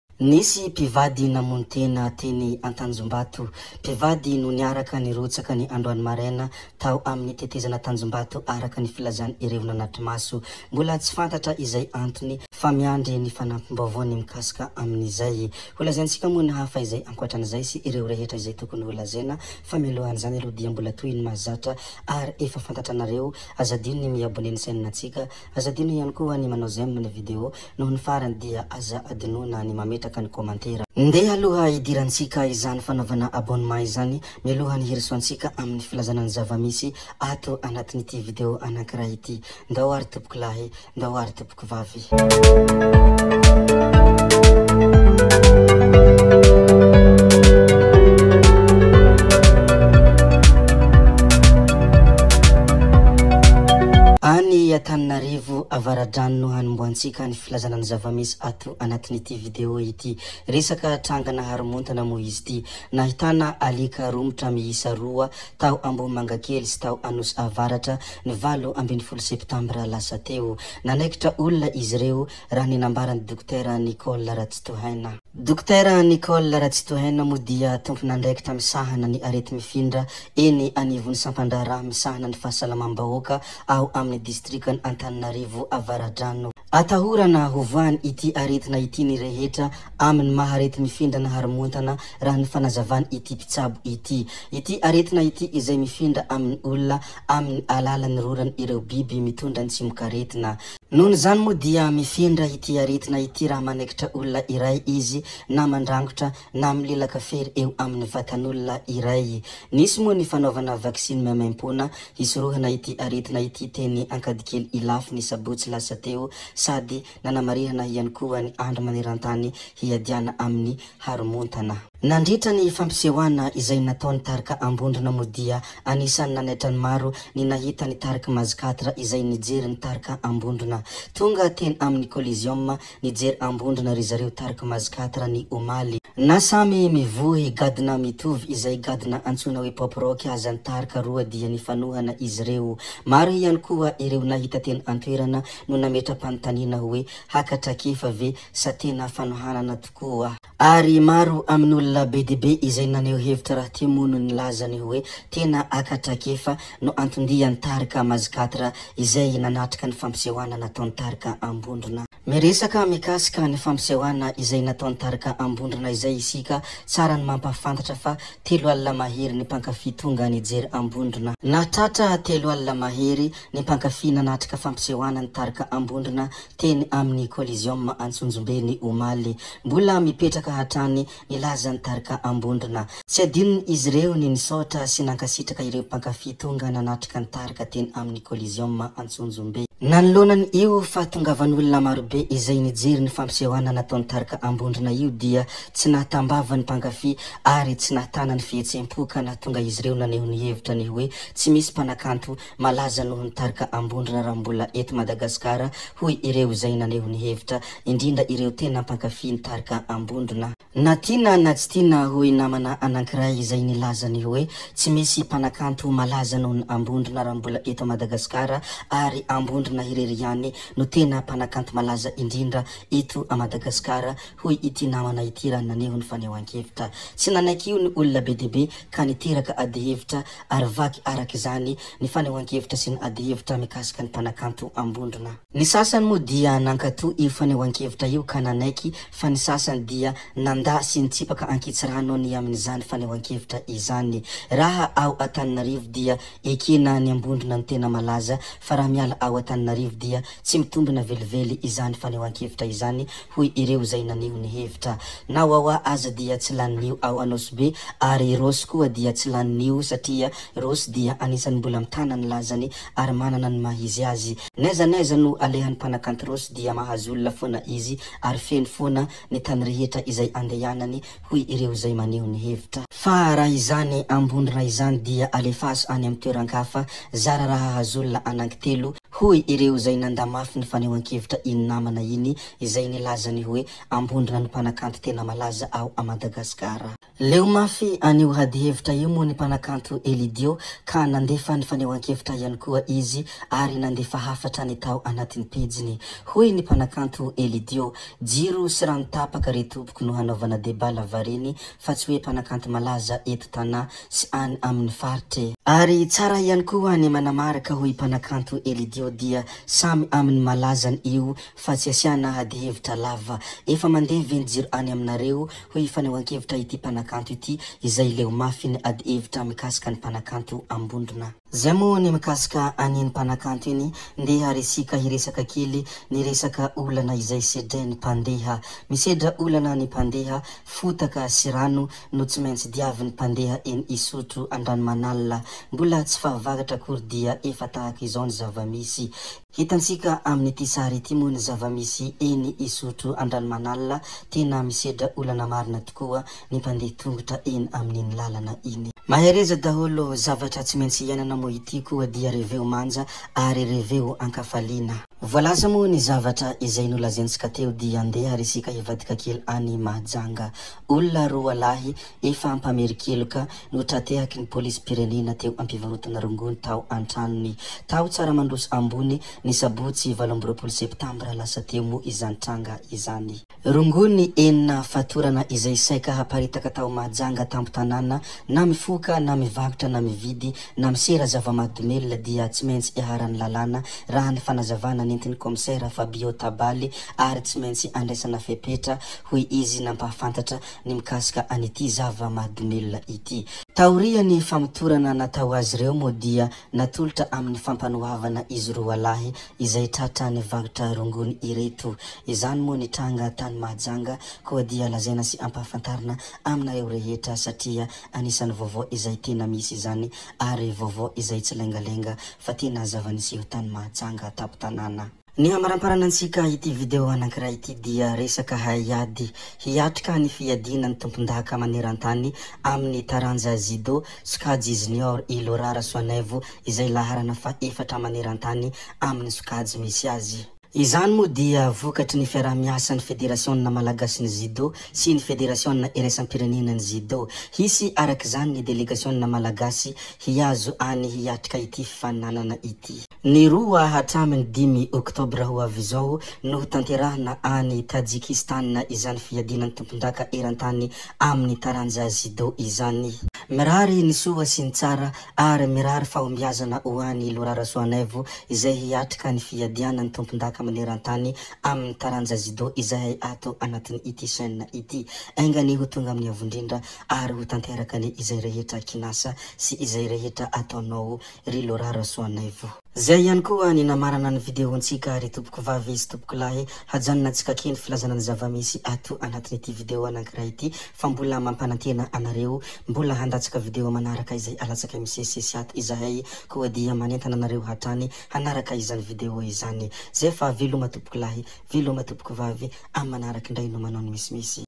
AMBONDRONA ZARA RAHA MAHAZO OLONA ANAN-KITELO RAHA ANY AMINY TOEN-KAFA ( Nouveautés gasy 2024 )
OLONA MAHERINY 3 ALINA NO TONGA NIJERY NY FAMPISEHOANA NATAONY AMBONDRONA TENY AMINY COLISEUM ANTSONJOMBE NY OMALY . TSY NAHITANA VAVA ARAK’IZANY NY MPANKAFY AMBONDRONA KA NITENY HOE TSY MISY MPANAKANTO MALAZA NOHON’NY AMBONDRONA RAHA MBOLA MADAGASCAR ARY IZY NO MALAZA INDRINDRA HOY IZY . MARO NY TS NANAIKY IZANY KA NITERAKA ADY HEVITRA BE DIA BE KA NAHATONGA SASANY NILAZA HOE RAHA MANDEHA ANY AMINY FARITRA IZANY AMBONDRONA IZANY DIA ZARA RAHA MAHAZO OLONA TELO.